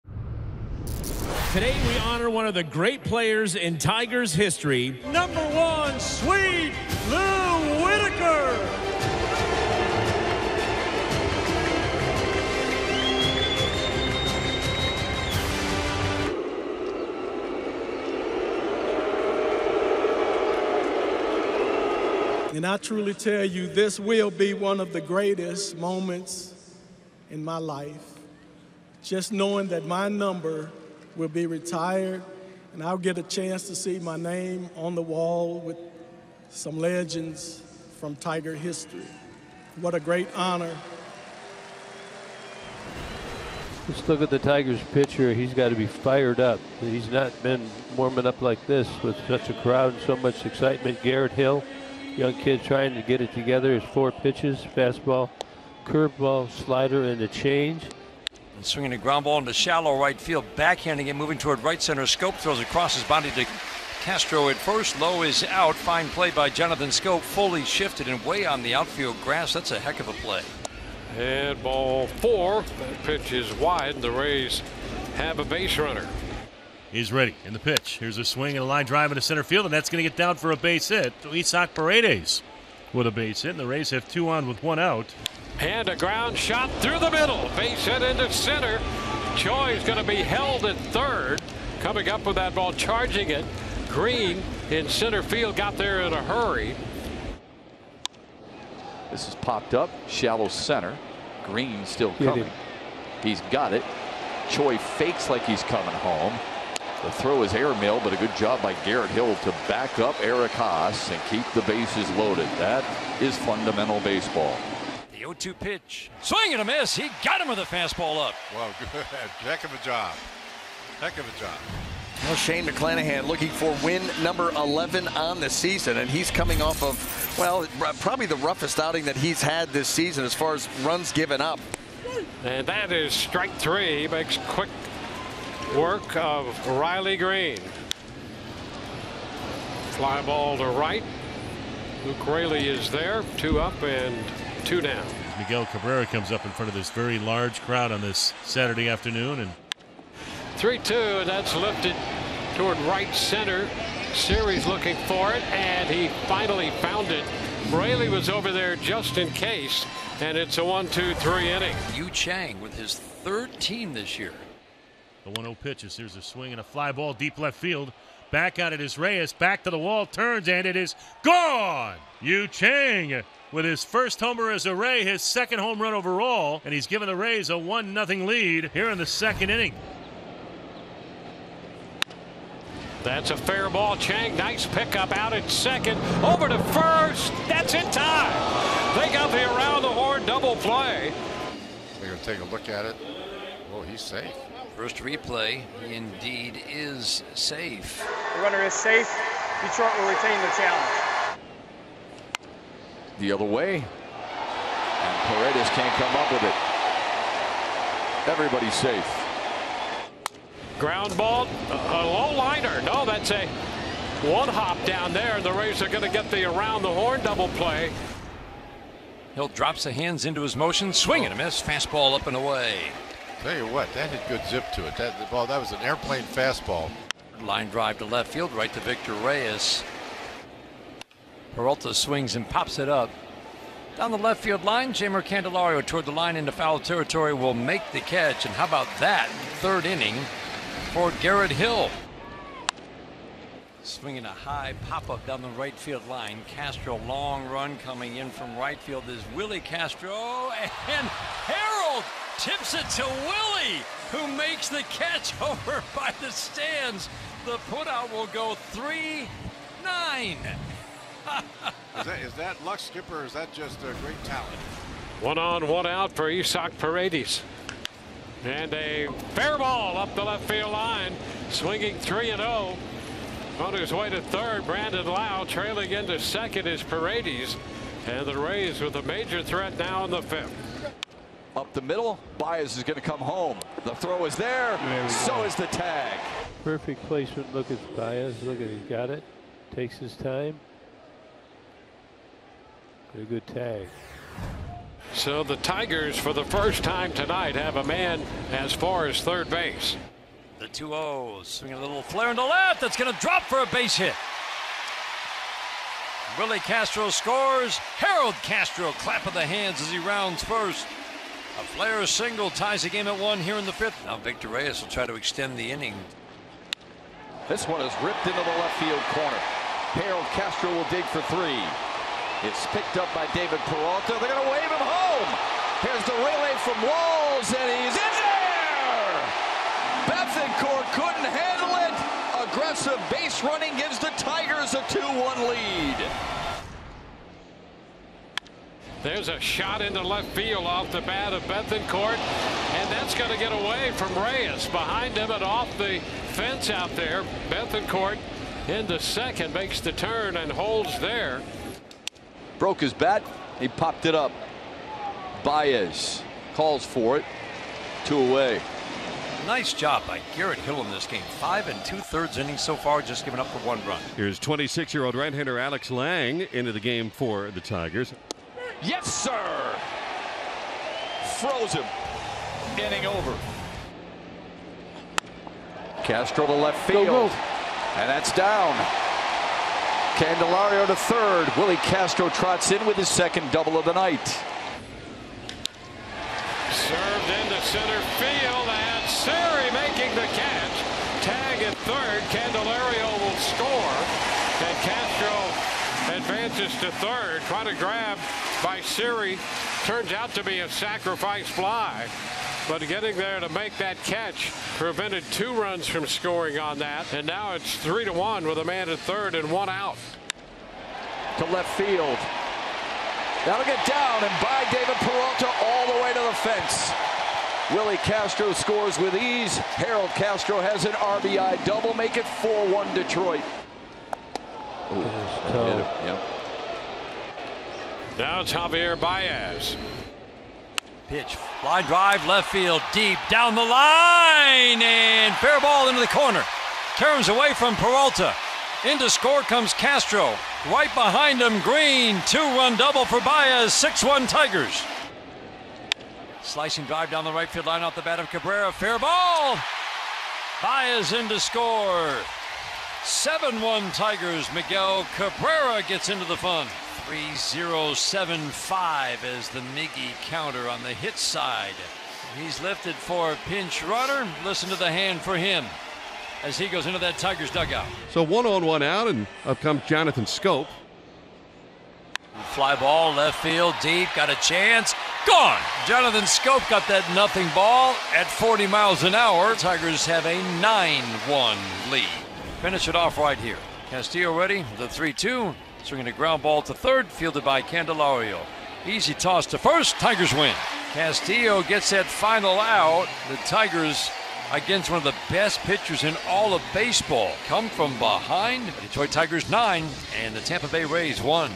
Rays vs. Tigers Game Highlights (8/6/22) | MLB Highlights
Rays vs. Tigers full game highlights from 8/6/22